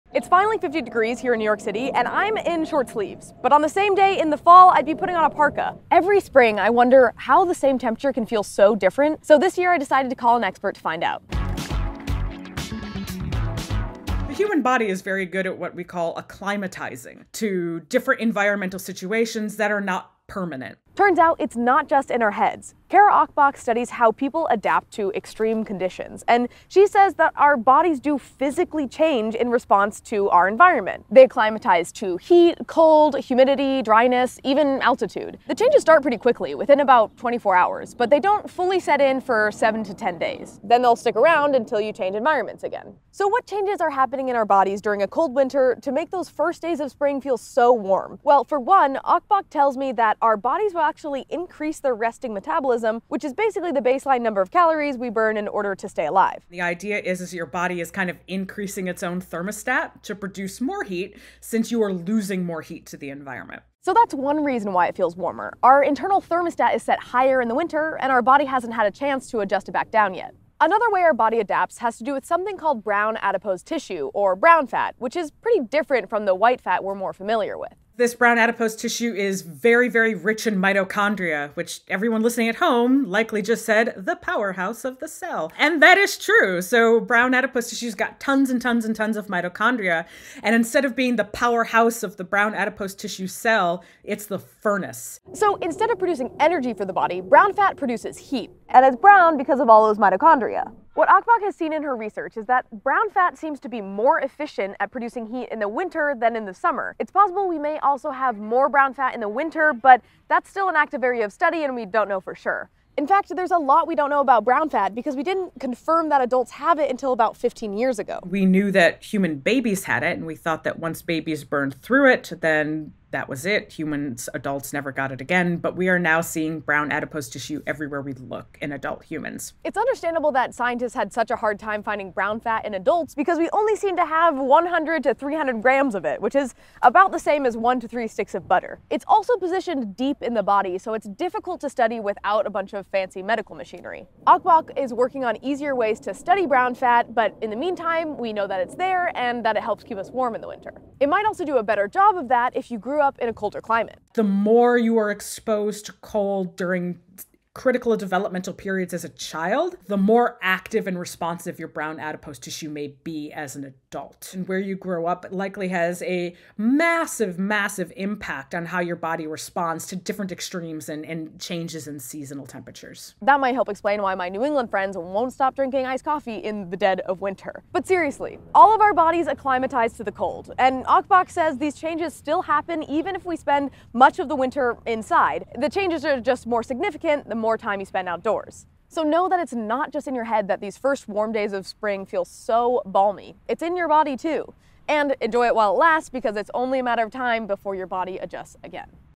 How the seasons actually change your body
Why is 50 degrees balmy in the spring and chilly in the fall? It turns out there are real, physiological reasons why the same temperature can feel so different depending on the season. Today we dive into the science of how our body “acclimatizes” to a cold winter.

Credits:
Hosted and reported by Kelso Harper
Additional reporting by Allison Parshall
Filmed by Carin Leong
Edited by Kylie Murphy
Additional imagery from Getty Images, Cara Ocobock
Music from Triple Scoop Music